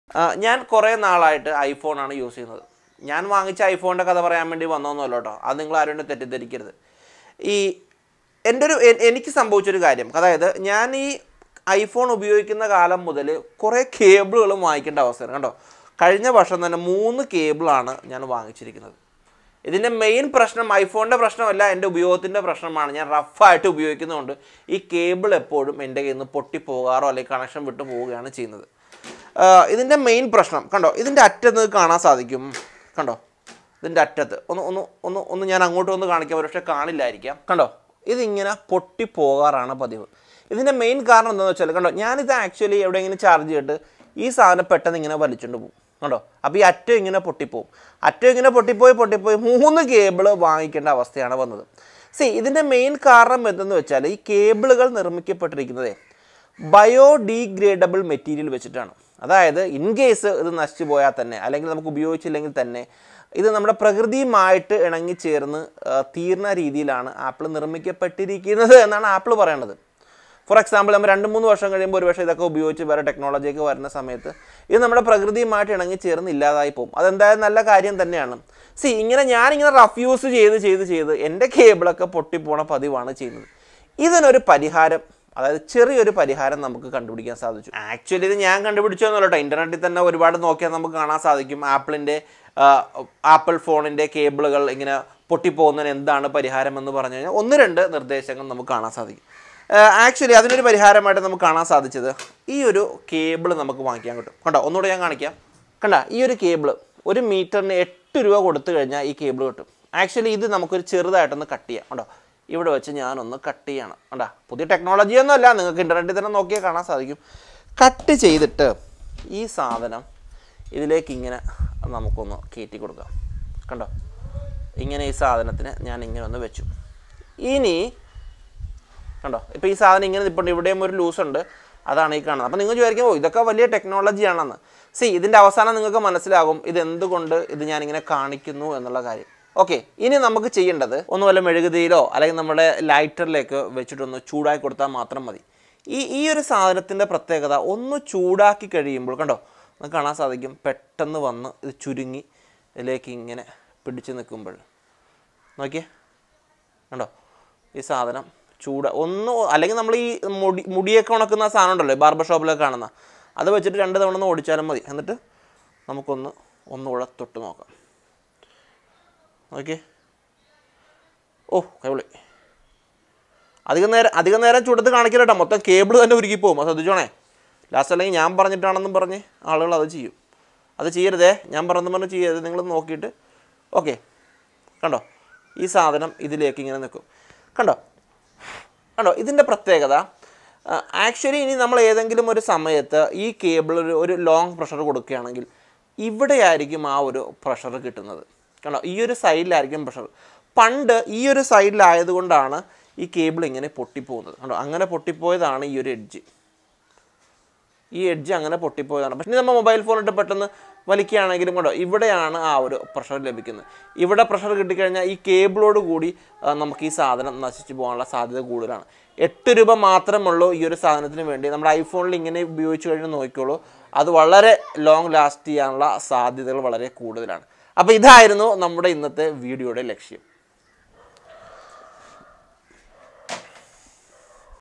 How to Solve i phone Cable Problem Malayalam
In this video, I explain how to solve I phone cable braking issues